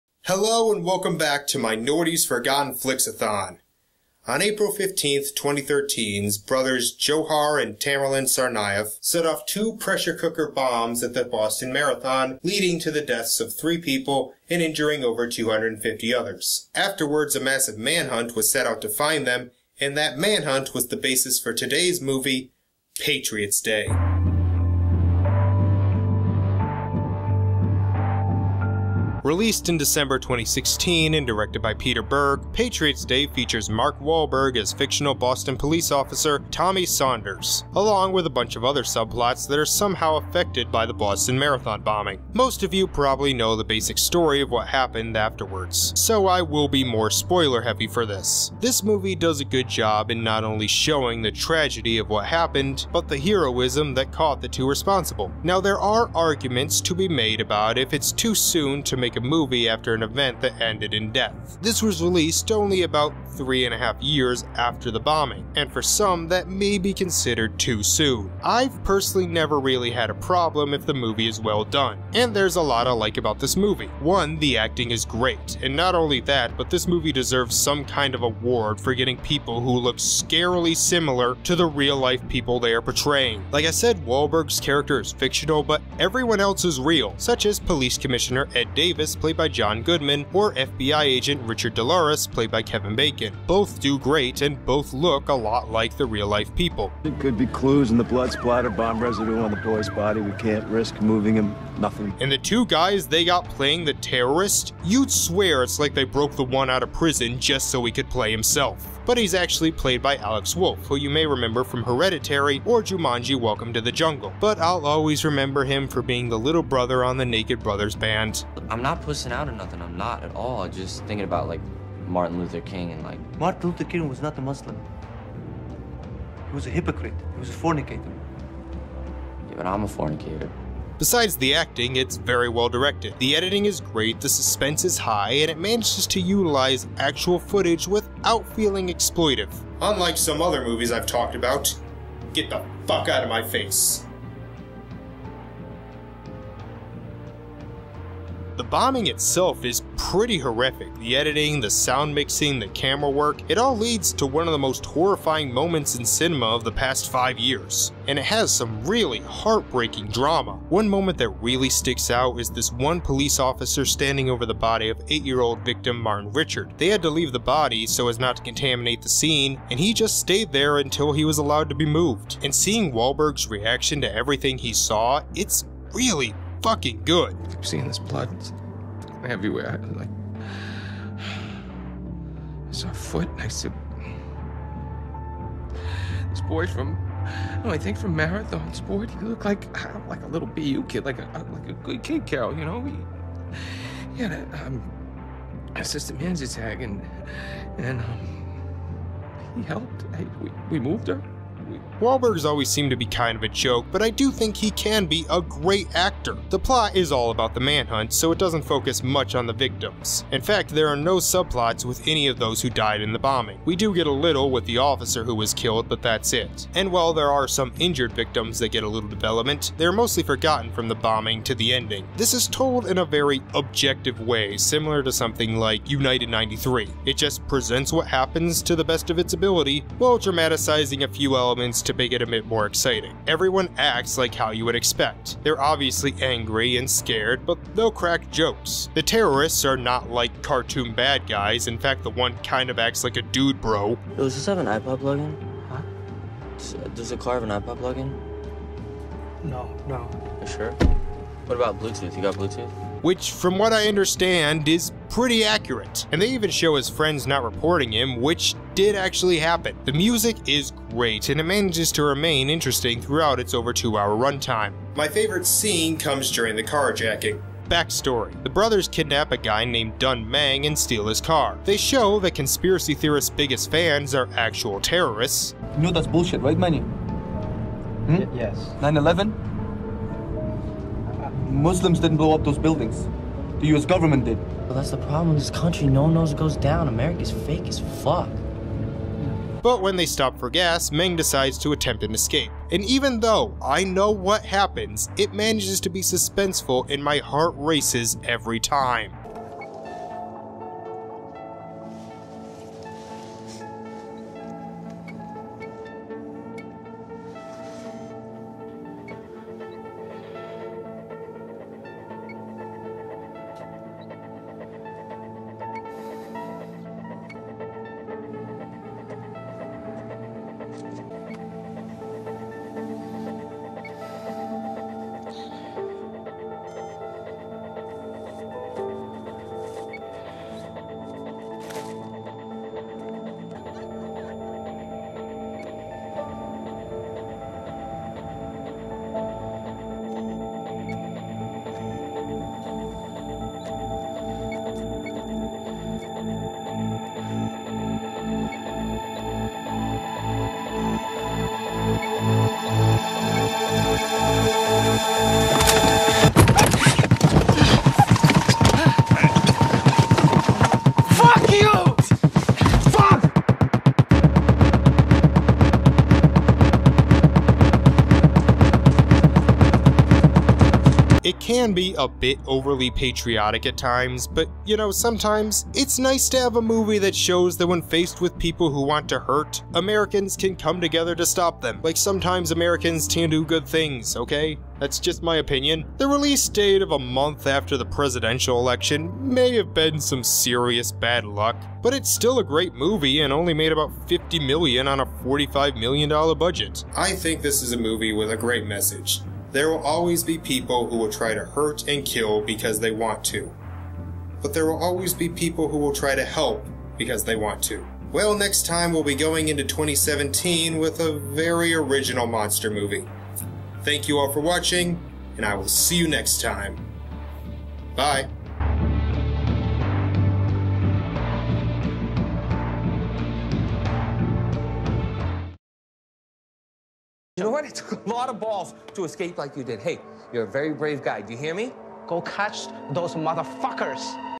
Norty's Forgotten Flicks: Patriot's Day
In 2013 two terrorists killed three people at the Boston Marathon. In 2016, the director of Battleship made a movie about it. don't worry it's really good.